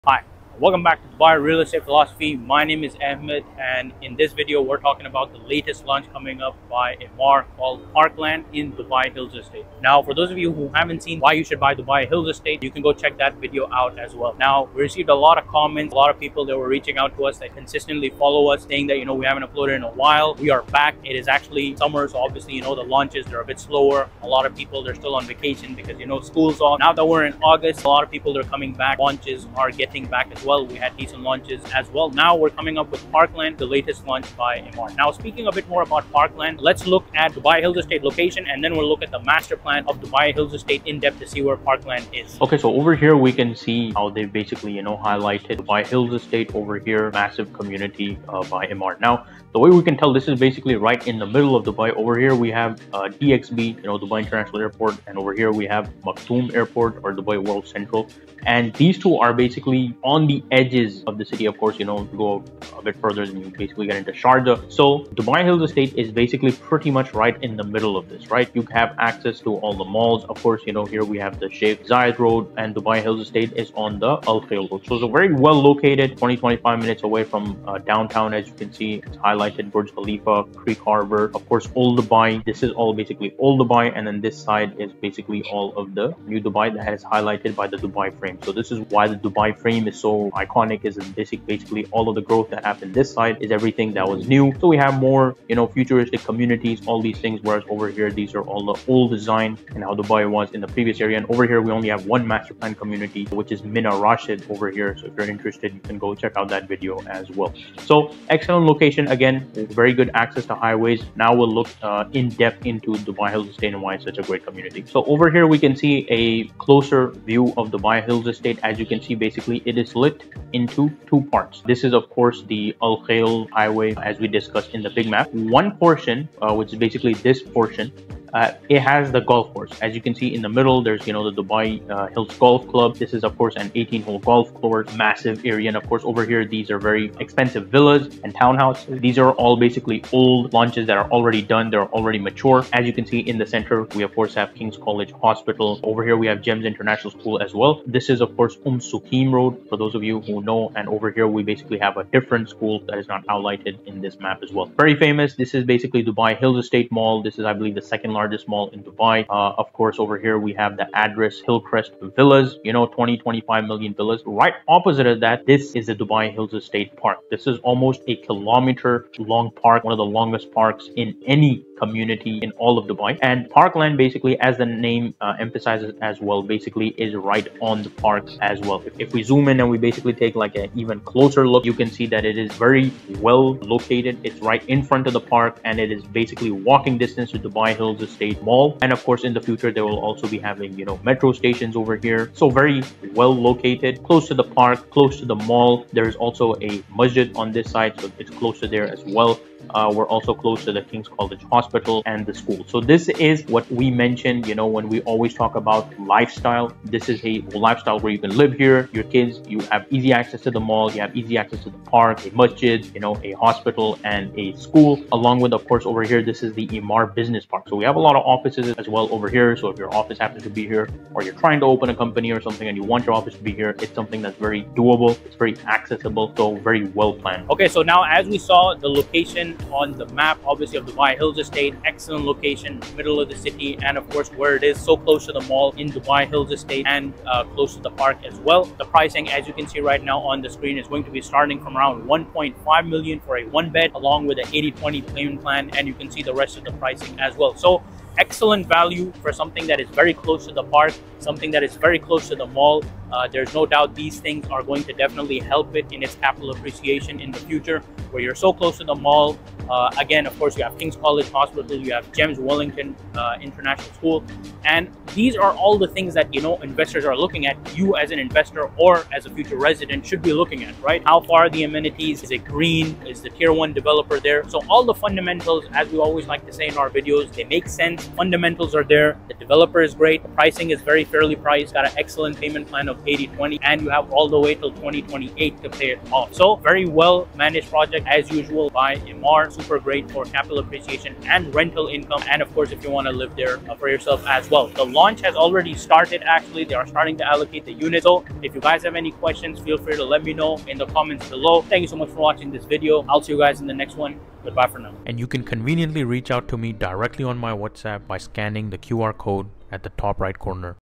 Latest Launch by EMAAR - Parkland review - Closest to Dubai Hills Mall & Park - Dubai 2024
Introducing Parkland by EMAAR, the latest launch located just steps away from Dubai Hills Mall and the park. Experience luxury living in this prime location, offering modern homes with easy access to top-tier amenities and green spaces. Join us for an exclusive tour of Parkland and discover why this development is the perfect blend of convenience and luxury in Dubai.